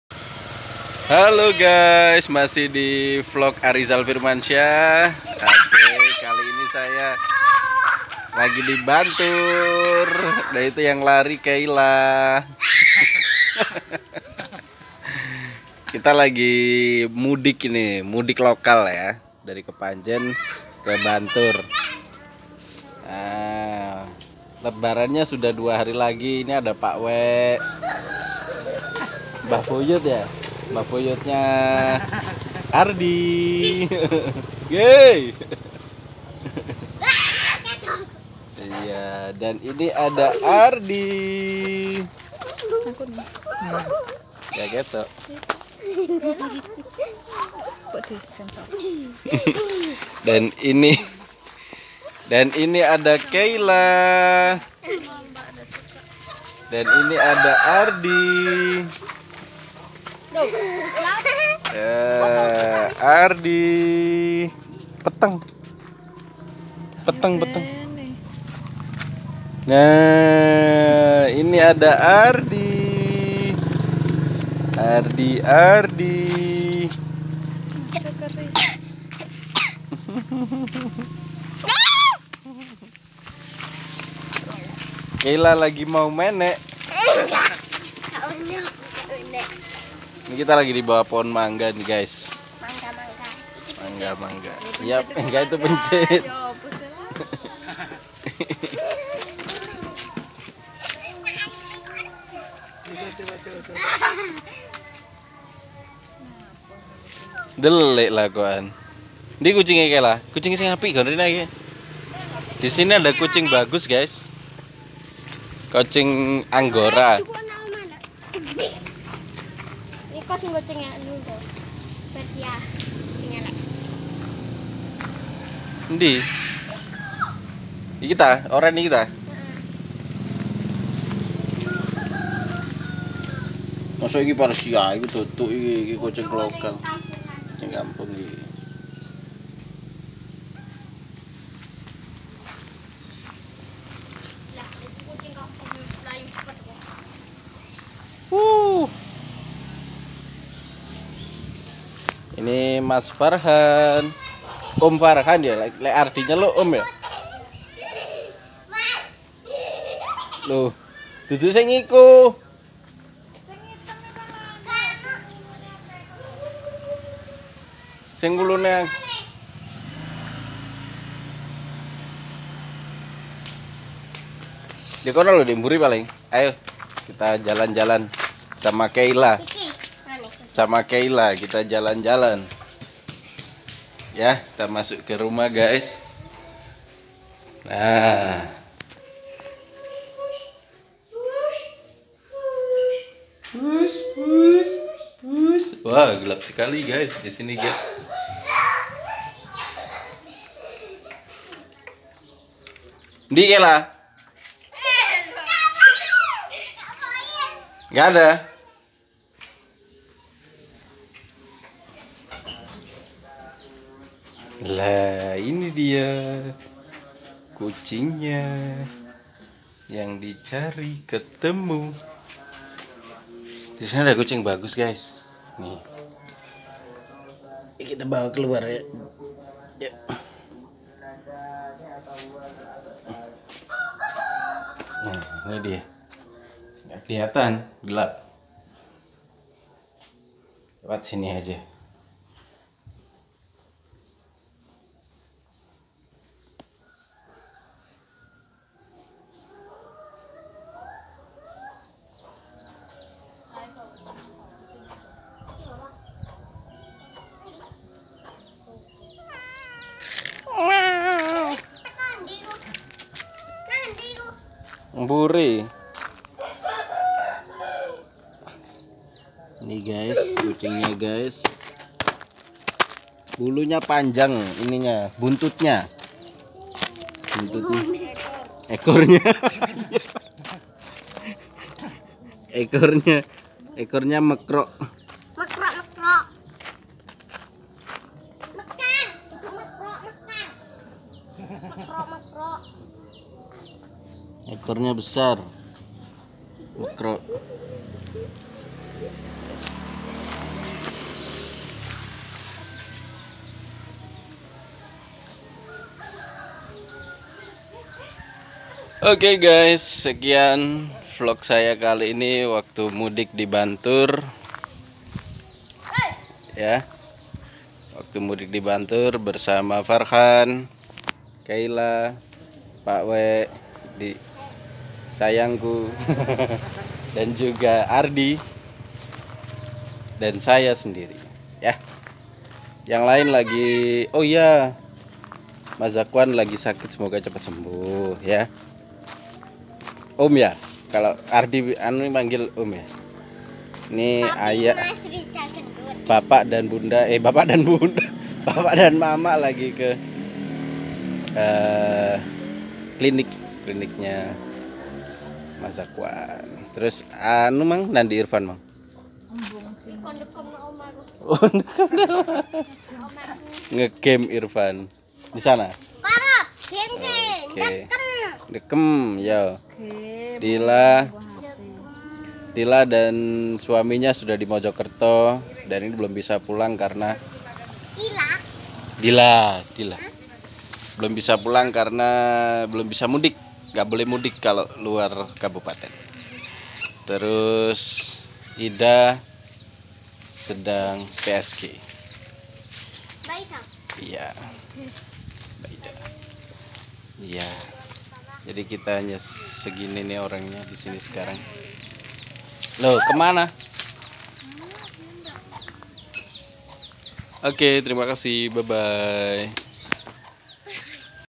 VLOG : MUDIK H-2 LEBARAN 2021
Dua hari sebelum Hari Raya Idul Fitri, saya bersama keluarga mudik alias pulang kampung ke Bantur, Malang, Jawa Timur.